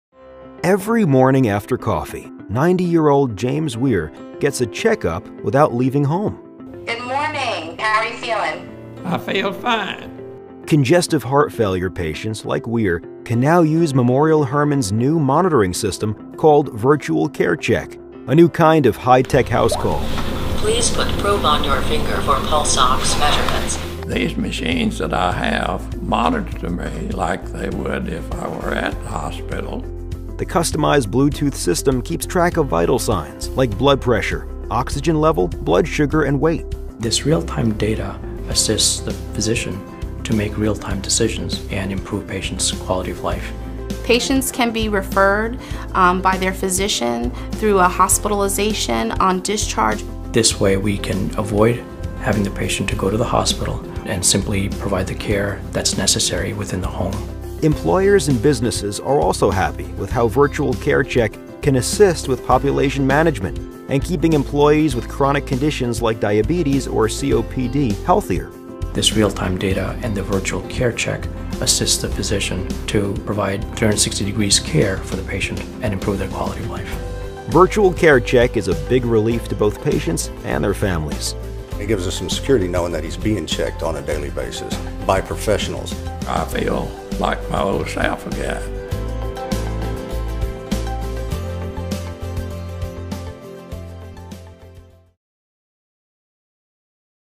Nonin Medical OEM pulse oximetry remote patient monitoring solutions
Learn how Memorial Hermann Hospital helps manage costs while improving the quality of one patient's life with the hospital's Virtual Care Check solution. The solution features Nonin Medical OEM pulse oximetry integrated into VIvify Health's remote patient monitoring solution.